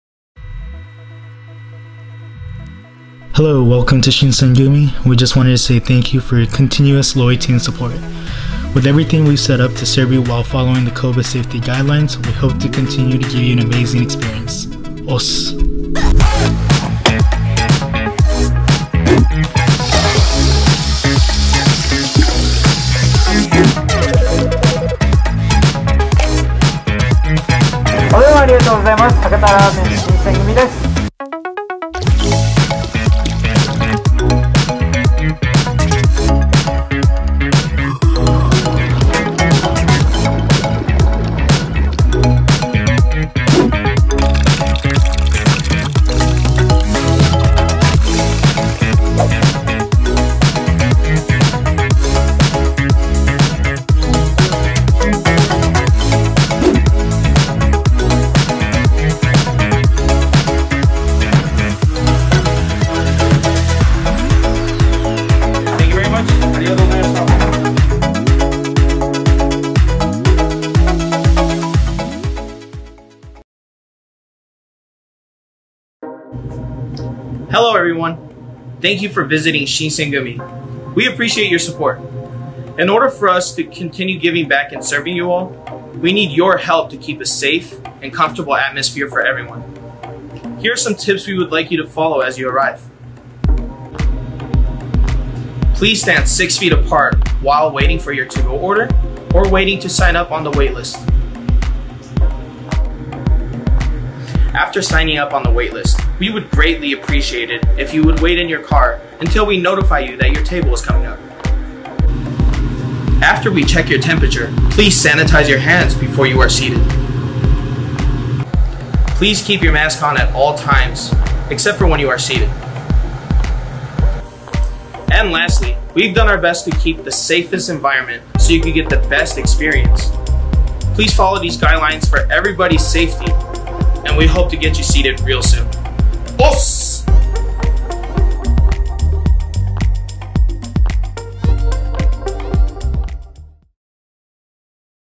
Shin-Sen-Gumi COVID-19 Dining Guidelines
We at Shin-Sen-Gumi thank you for your continue support.
We are committed to serving you and thank you for being a loyal guest.
Please follow the following dining guidelines and enjoy dining at Shin-Sen-Gumi Restaurants!
Video by Ryo Tamada